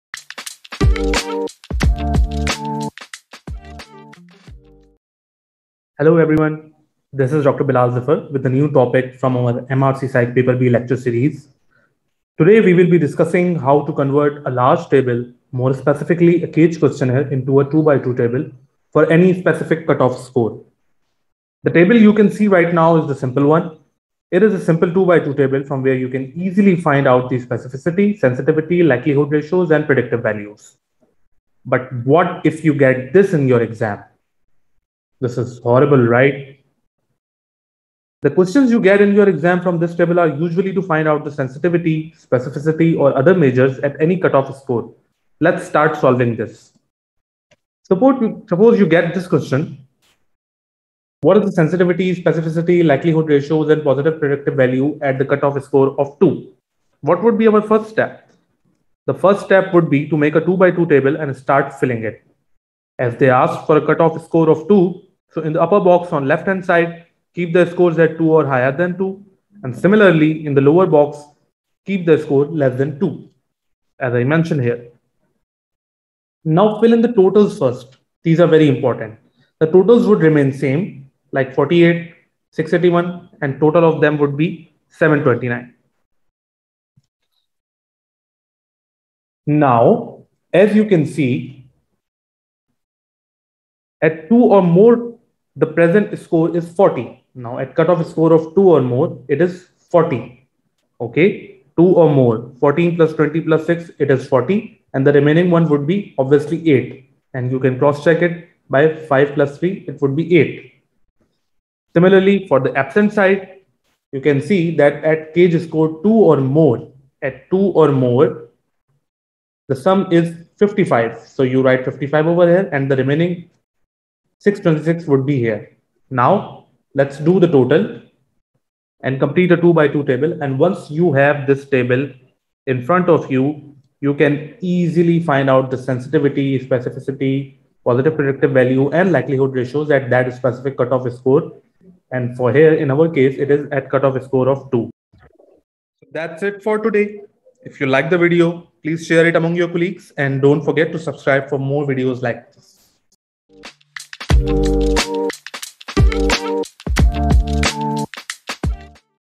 MRCPsych Paper B Stats | How to solve questions from CAGE questionnaire?
Finding out Sensitivity, Specificity, Likelihood ratios, and Predictive values from a 2x2 table is pretty straightforward. However, it gets quite tricky if you have a large table (like a CAGE questionnaire).

In this video, you'll learn how to convert a large table into a simple 2x2 table in order to find out different measures.